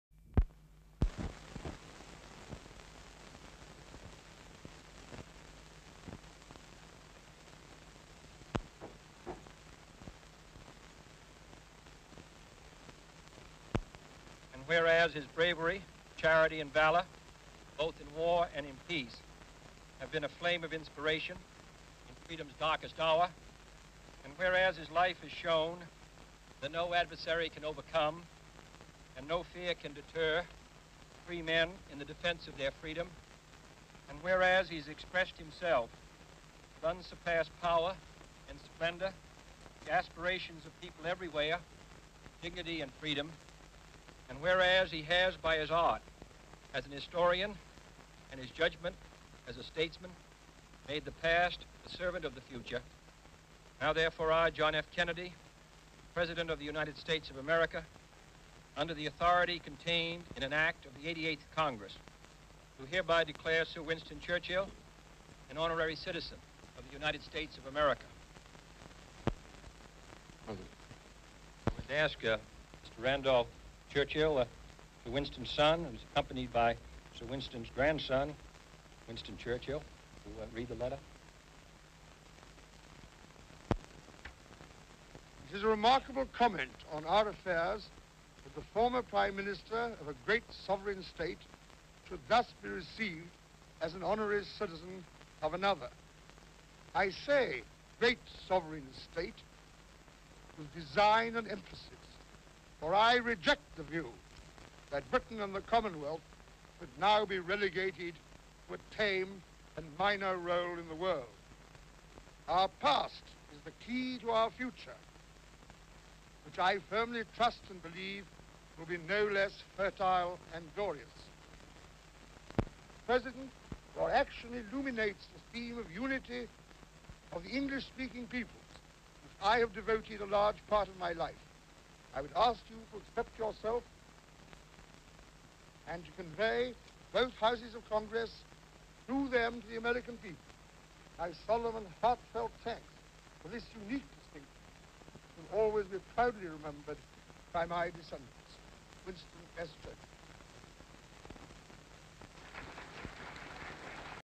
John F Kennedy makes Winston Churchill an honorary American citizen
Winston Churchill Becomes an Honorary American Citizen.